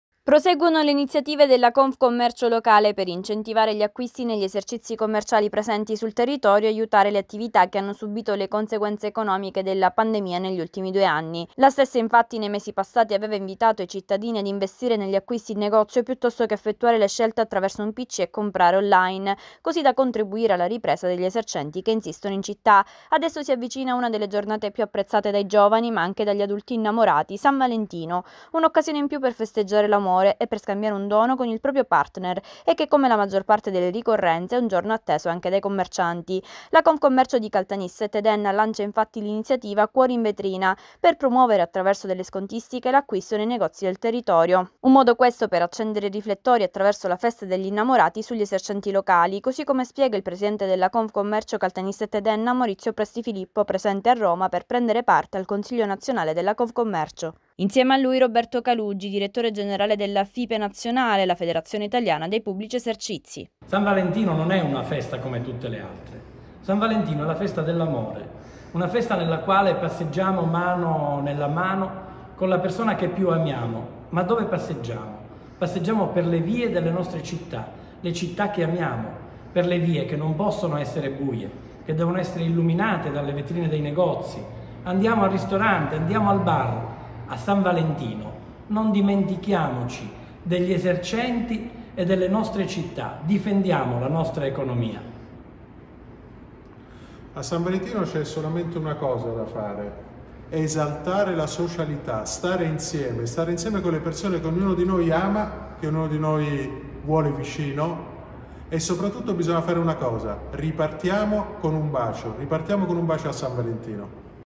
Caltanissetta - “Cuori in vetrina” per incentivare gli acquisti - TFNWEB.IT
Iniziativa della Confcommercio di Caltanissetta e Enna per incentivare  gli  acquisti durante la festa di San Valentino. Sconti per chi compra nei negozi convenzionati